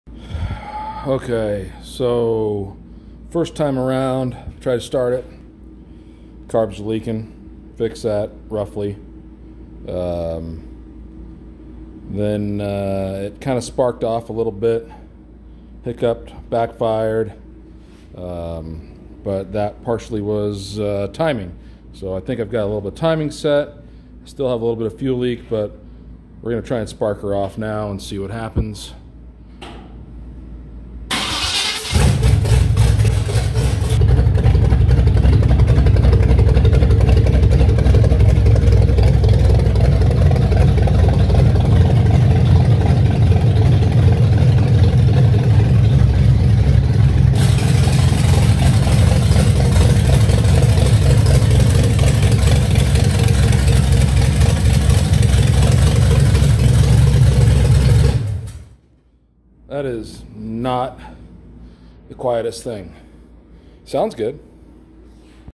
Rat Rod first start
firing up the 27 chevy for the first time.